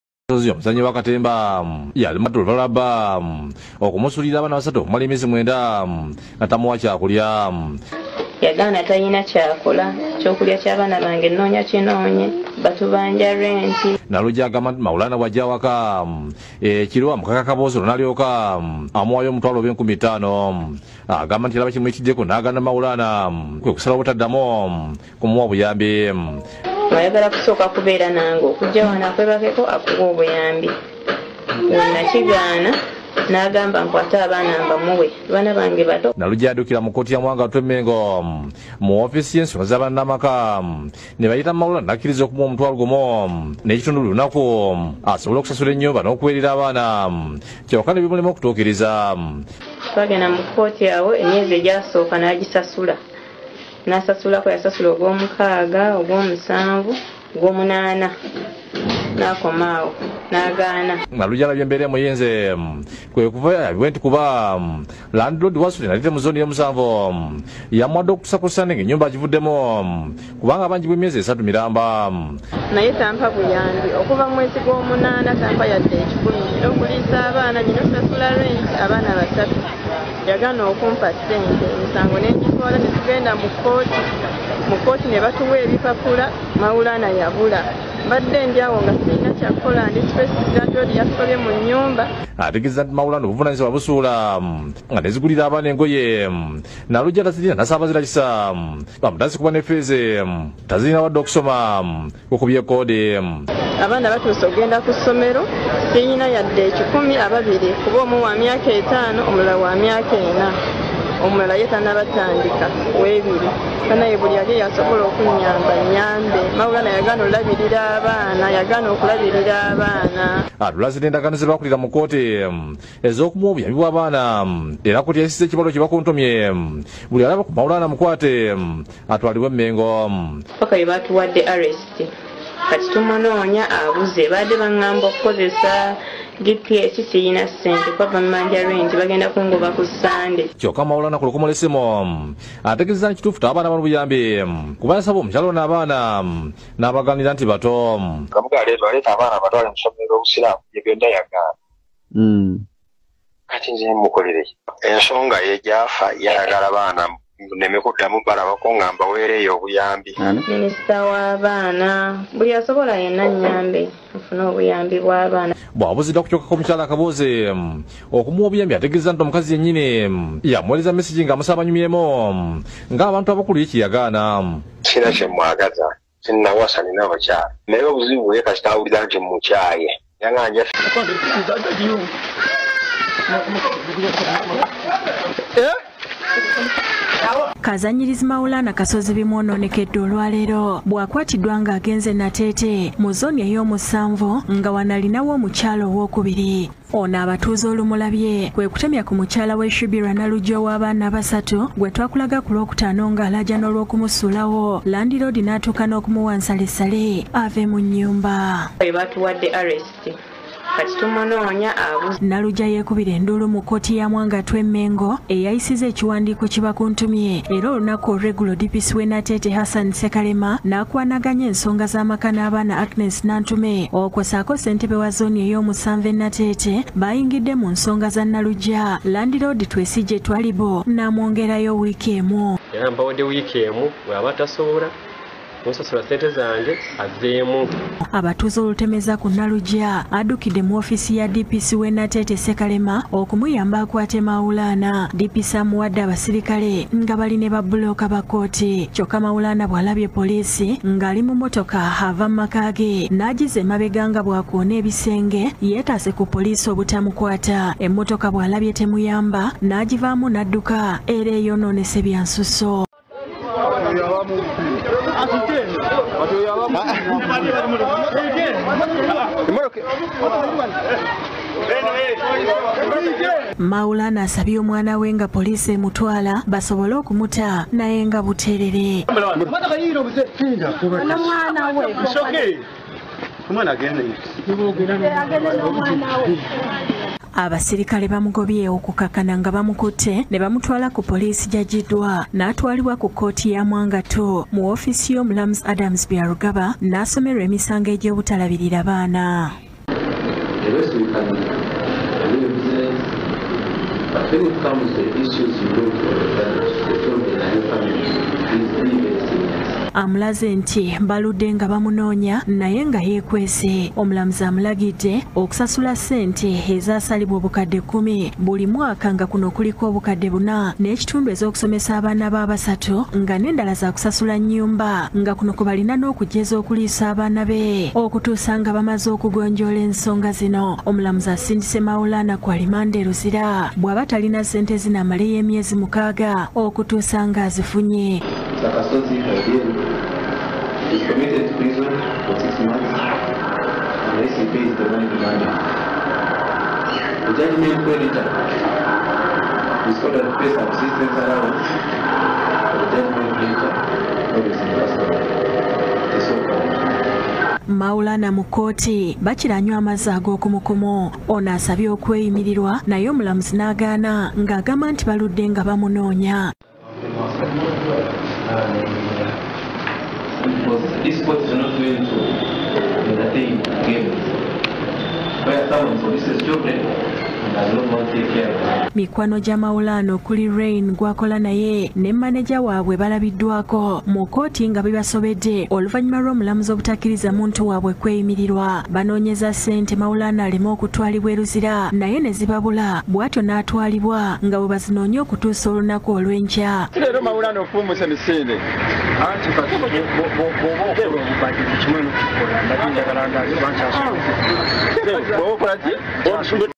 Poliisi ekutte Maulana olw’obutalabirira baana n’asimbibwa mu kkoot.
Poliisi ekutte kazannyirizi Maulana nga akabizi n’asimbibwa mu kkooti olw’obutalabirira baana